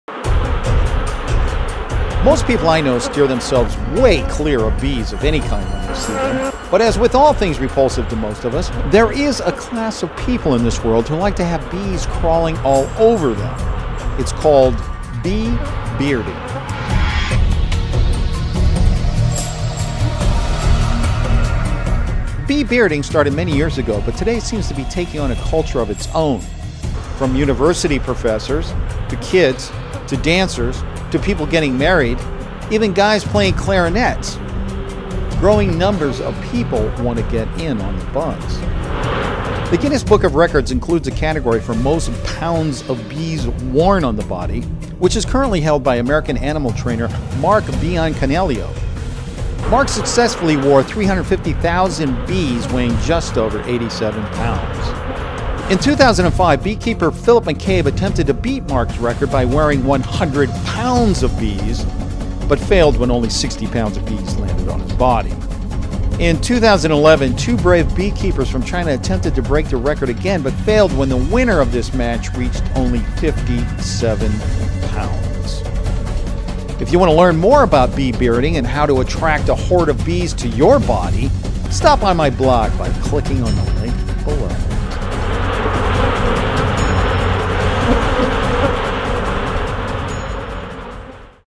20 People Covered in Bees
Bee bearding is the practice of wearing several hundred thousand honey bees on the face, usually as a sideshow-type demonstration at agricultural shows. Hive bees are attracted into position by a queen in a small cage worn under the chin.

Though beekeepers since ancient times have allowed bees to rest on their bodies in order to demonstrate their rapport with the insects, the practice of congregating measurable quantities of bees on the face was initiated by Petro Prokopovych, a Ukrainian beekeeper, in the 1830s. The practice spread to various "freak" exhibitions at American carnivals by the end of the nineteenth century.

The Guinness Book of Records includes a category for "most pounds of bees worn on the body," which is currently held by American animal trainer Mark Biancaniello. Biancaniello successfully wore 350,000 bees, weighing just over 87 pounds, during a 1998 broadcast of the Guinness World Records: Primetime television show.

A 2005 attempt to break the record by Irish beekeeper Philip McCabe, who was to wear a full one hundred pounds of bees, failed when only 60 pounds of bees landed on his body. McCabe was using Irish black bees, which are larger than Italian honey bees, so fewer bees (around 300,000) would have been required to reach 100 pounds than it took Biancaniello to reach 87 pounds. Bee bearding records are calculated by weight, not number of bees, because of this variation across honey bee strains.

Due to ever-increasing competition to break the record, most advanced attempts at bee bearding now involve bees covering the entire face, torso, back, and arms of the participant in order to provide sufficient surface area for over eighty pounds of bees to land. Nevertheless, the name "bee bearding" is still used.

In the 2008 The Simpsons episode "The Burns and the Bees", Lisa Simpson wears a beard of bees to help the local population of bees to flourish.

The episode "Aptitude" on the series The Suite Life of Zack & Cody shows Zack looking at a video called "Beard of Bees" on a YouTube-like website.

In Bee Movie, as a video clip on a bee news program, a crowd of bees chants "No More Bee Beards! No More Bee Beards!"

In the King of the Hill episode Mutual of Omabwah, Dale Gribble wears a bee beard.

In the 2000 Malcolm in the Middle episode "The Bots and the Bees", Hal wears a beard of bees as a result of his battle robot.